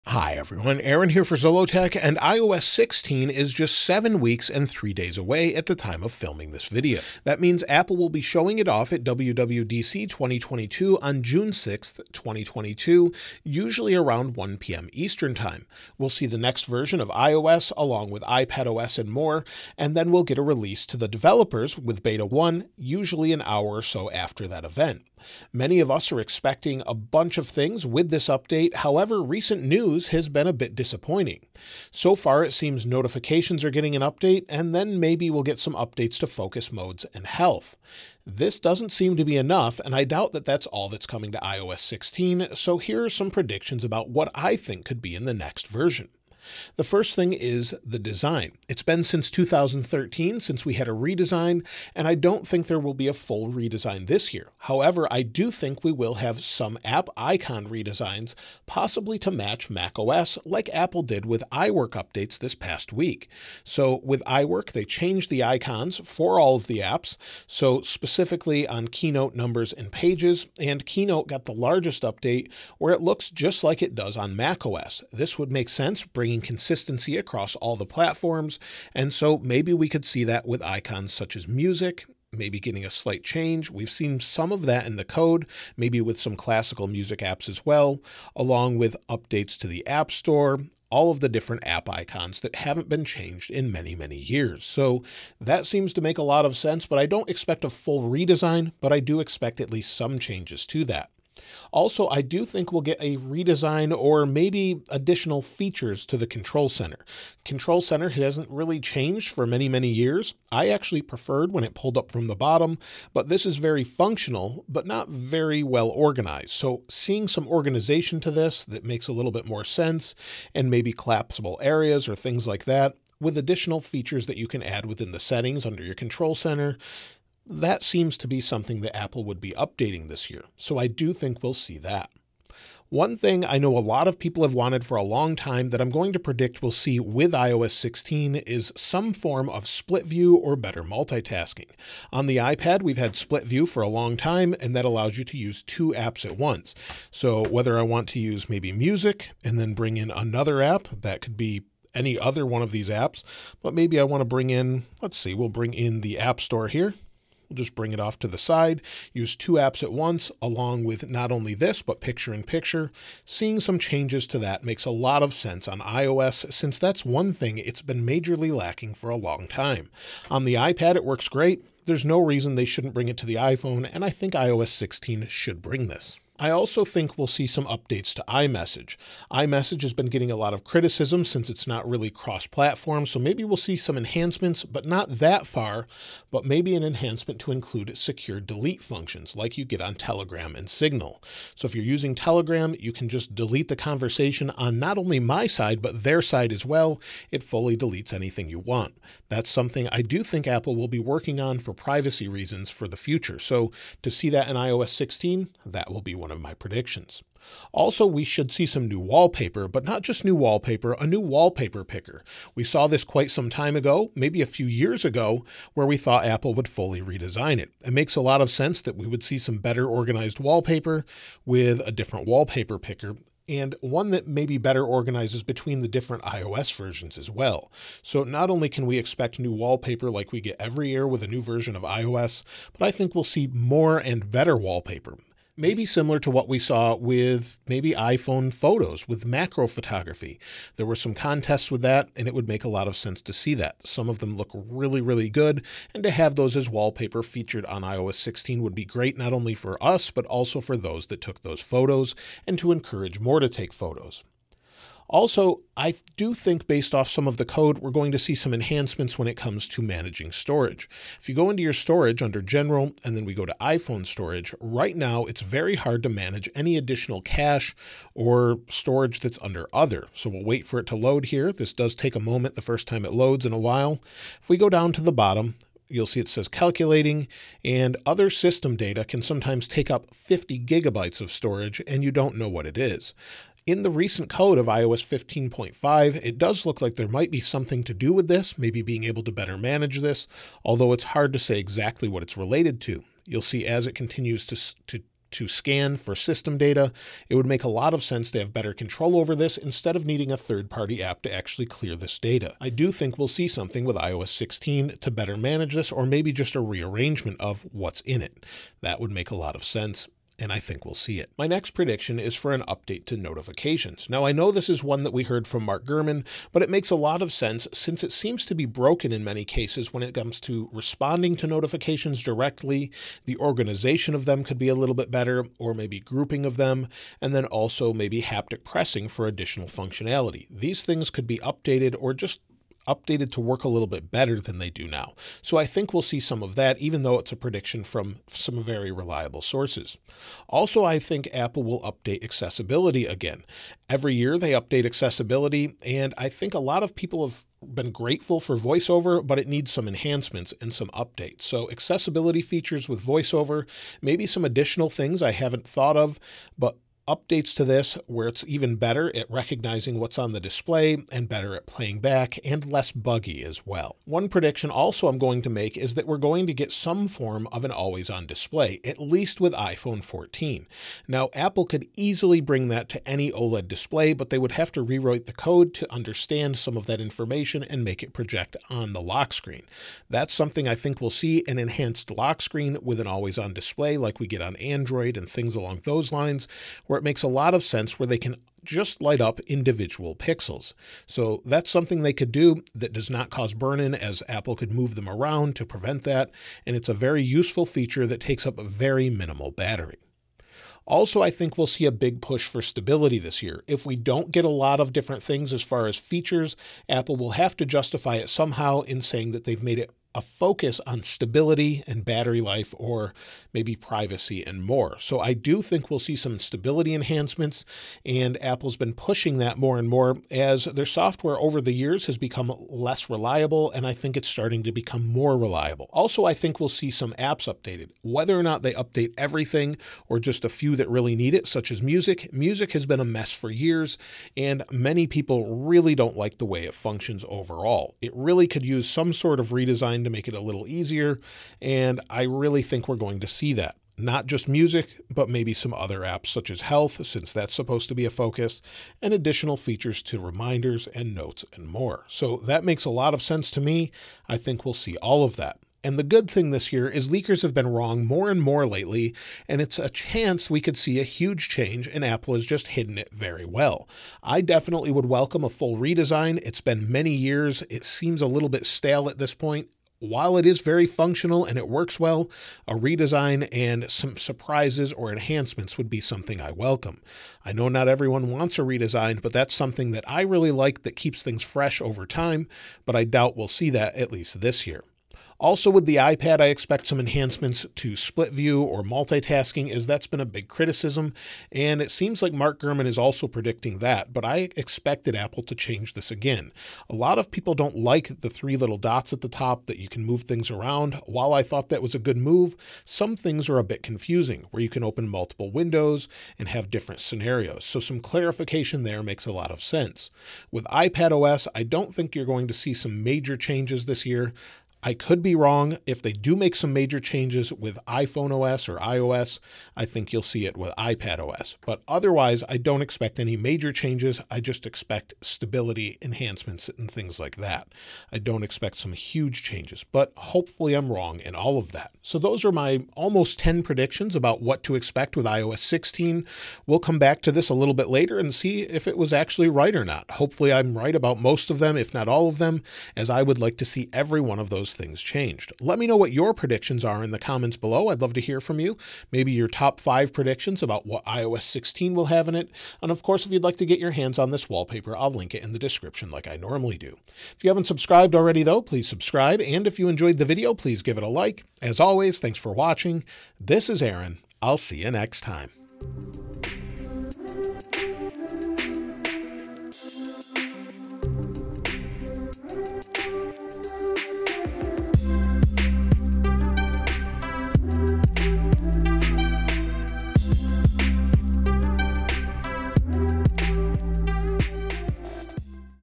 iOS 16 Release Date and What We Could See
iOS 16 - Release Date, Features, Updates and Changes

iOS 16 is six weeks from being shown for the first time at WWDC22 where Apple will show iOS 16, iPadOS 16, macOS 13, watchOS 9, tvOS 16 and more.  WWDC22 is on June 6, 2022 and after the keynote Apple is planning to release iOS 16 Beta 1 to developers.

iOS 16 could finally have new icons, split screen multitasking, iMessage updates and more.  This is my prediction about what features and updates could be in iOS 16. Also some iOS 16 Rumors and when to expect iOS 16 to be released.

***Chapters***
00:00 - Introduction
00:07 - iOS 16 Release Date
01:32 - iOS 16 Features
02:56 - Messages
04:33 - Storage
05:11 - Notifications
06:03 - Display
07:05 - App Updates
08:05 - iOS 16 a big surprise
09:25 - Outro